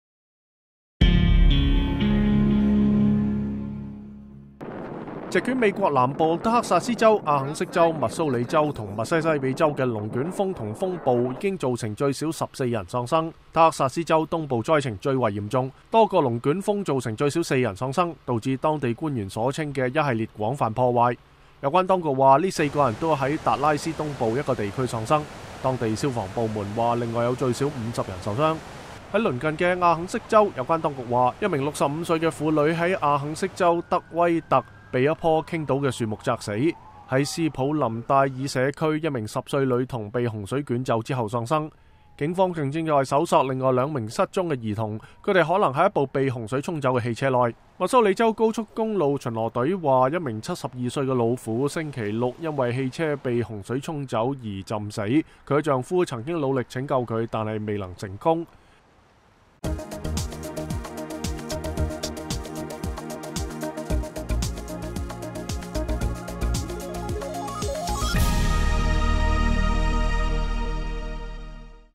2017-05-01 美國之音視頻新聞: 美國龍捲風洪水肆虐致多人喪生 (粵語)
席捲美國南部德克薩斯、阿肯色、密蘇里和密西西比州的龍捲風和風暴已經造成至少14人喪生。德克薩斯州東部災情最為嚴重，多個龍捲風造成至少4人喪生，導致當地官員所稱的一系列廣泛的破壞。有關當局表示，這4人都是在達拉斯東部一個地區喪生的。當地消防部門說，另外至少有50人受傷。警方還在搜索另外兩名失蹤的兒童，他們可能在一輛被洪水沖走的汽車裡。